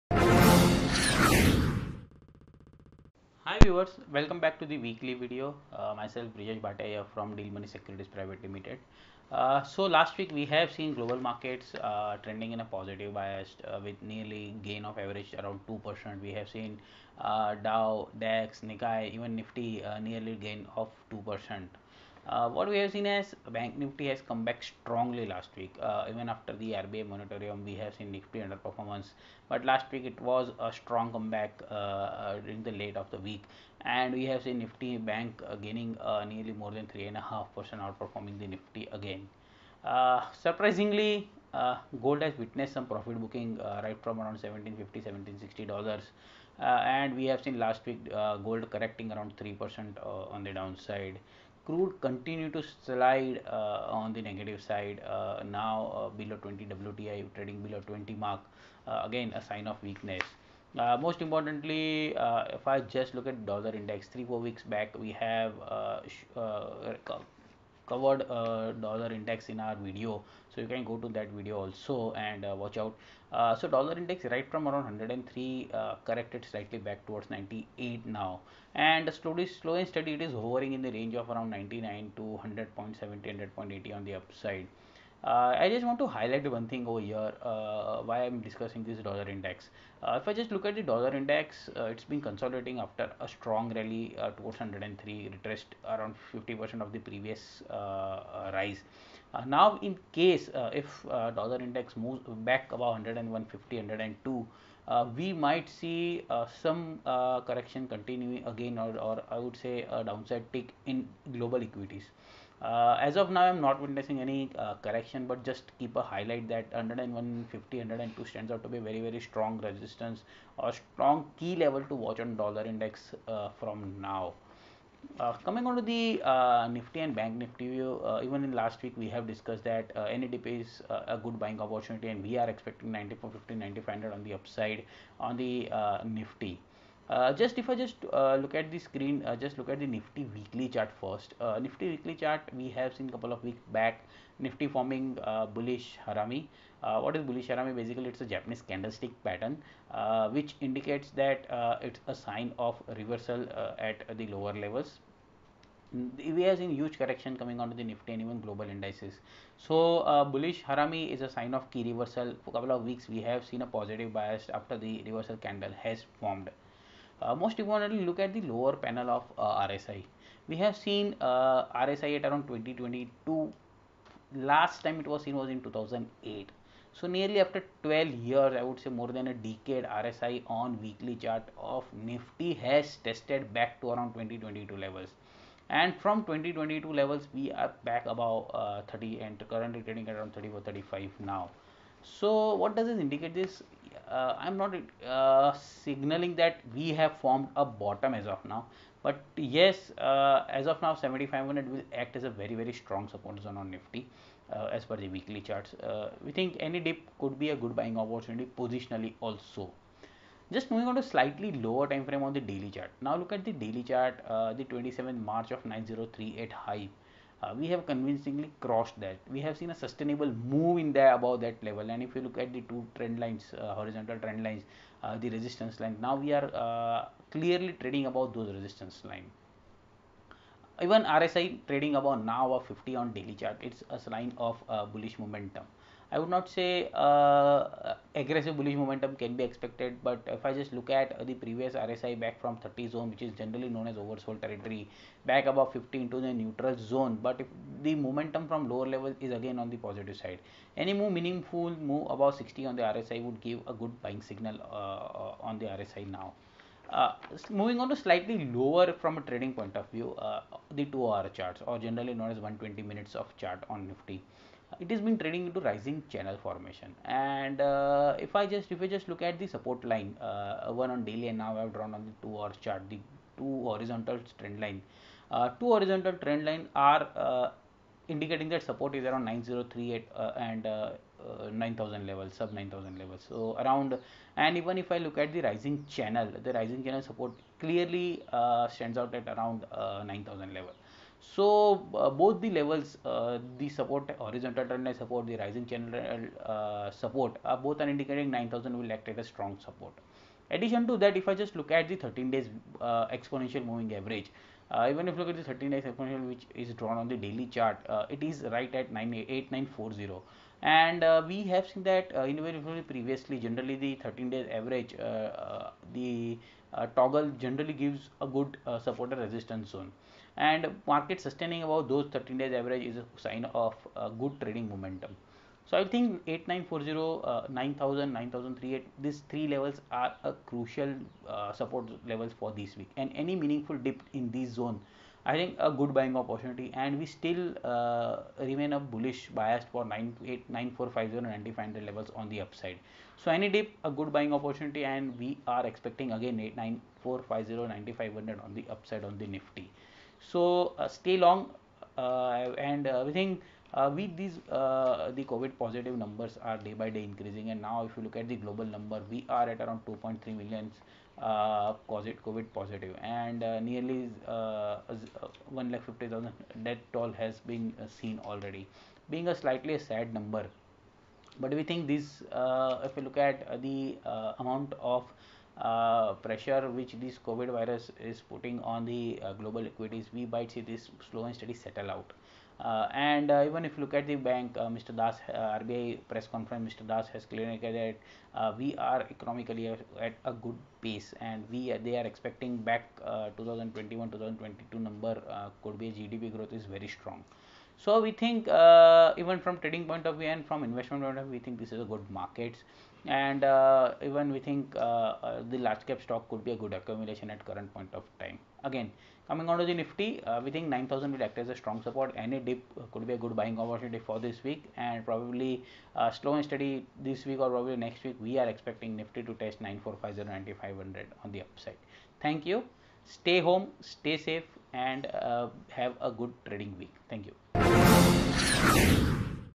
Nifty Weekly and Daily chart indicates positive trend to prolong By Mr. Brijesh Bhatia, Dealmoney
Nifty Weekly and Daily chart indicates positive trend to prolong By Mr. Brijesh Bhatia, Head of Research, Dealmoney Securities Pvt.